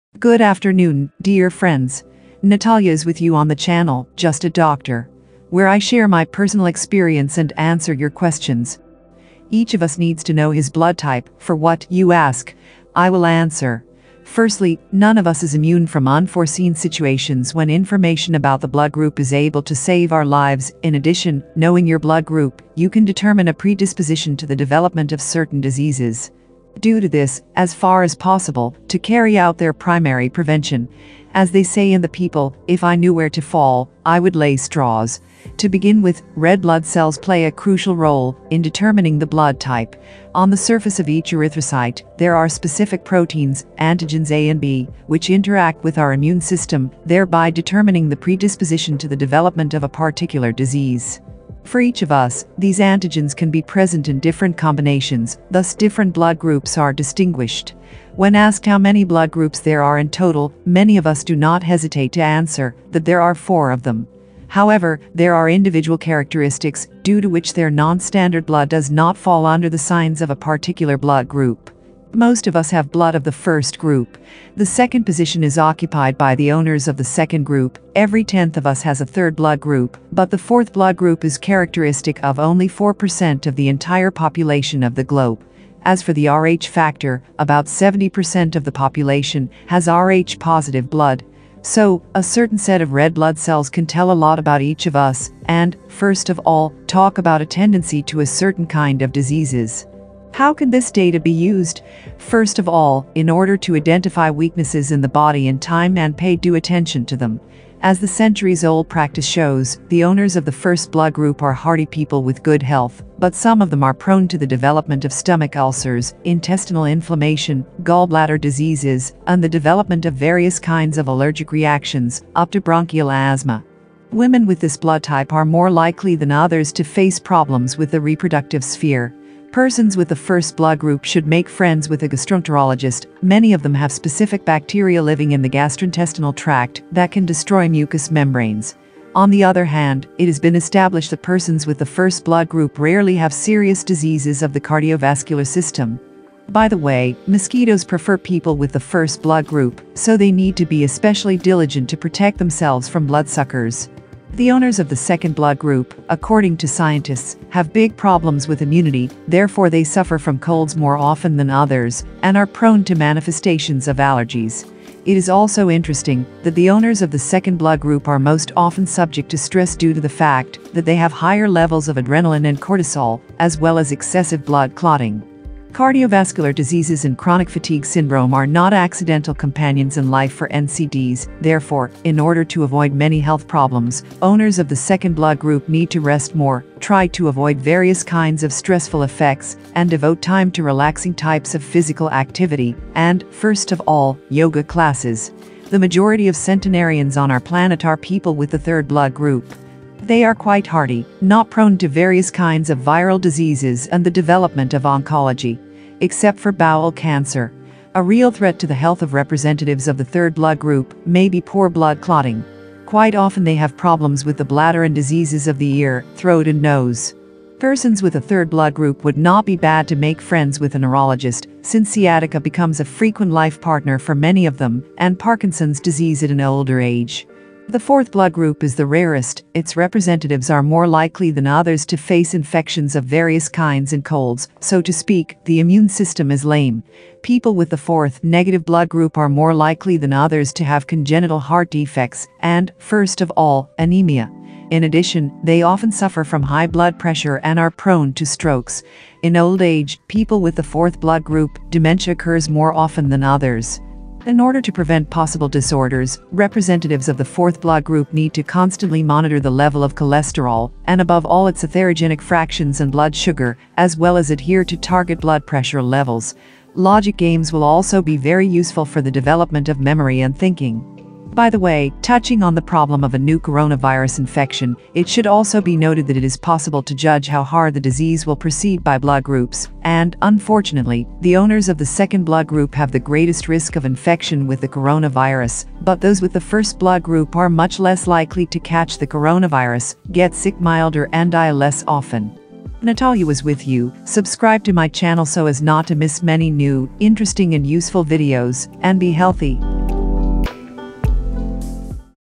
Blood type and diseases. Interesting facts about blood types. Blood type and COVID
A blood type (also known as a blood group) is a classification of blood, based on the presence and absence of antibodies and inherited antigenic substances on the surface of red blood cells (RBCs). These antigens may be proteins, carbohydrates, glycoproteins, or glycolipids, depending on the blood group system. Some of these antigens are also present on the surface of other types of cells of various tissues. Several of these red blood cell surface antigens can stem from one allele (or an alternative version of a gene) and collectively form a blood group system.

Each of us needs to know his blood type, for what, you ask. I will answer. Firstly, none of us is immune from unforeseen situations when information about the blood group is able to save our lives, in addition, knowing your blood group, you can determine a predisposition to the development of certain diseases. Due to this, as far as possible, to carry out their primary prevention. You will learn about this and much more by watching this video, your just a doctor.
Before applying the recommendations, be sure to consult a specialist!